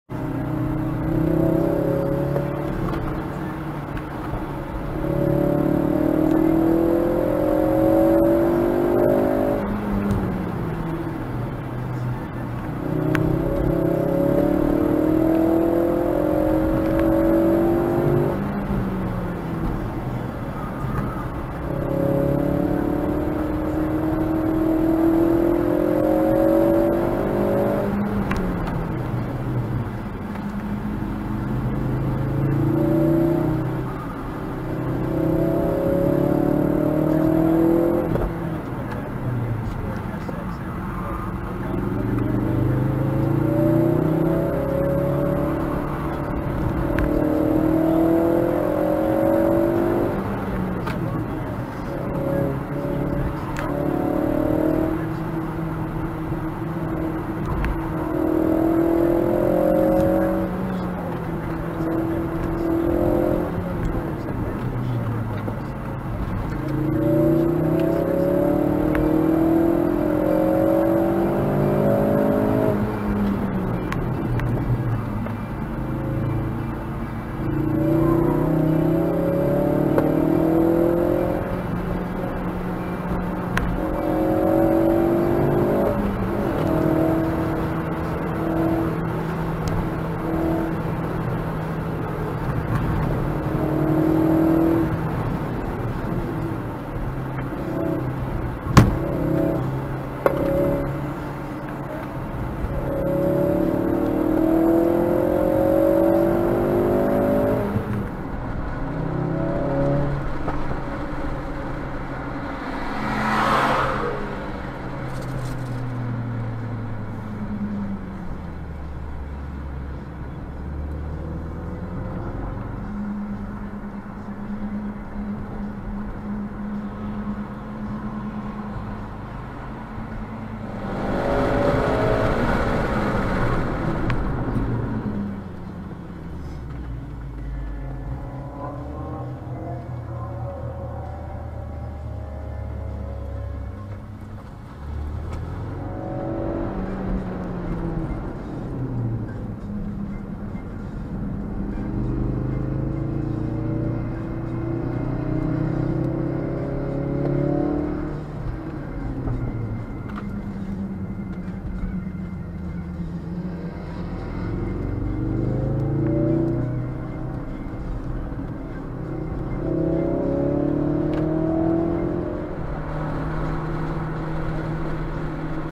2020 #1 Mosport DDT
My "fastest" lap for event #1 plus some extra practice content at the end.
2018 Focus RS w/ RE71R tires

First event at the track this year as things slowing open up. Everyone physically distancing. I am obviously very rusty as wasn't pushing hard enough all day.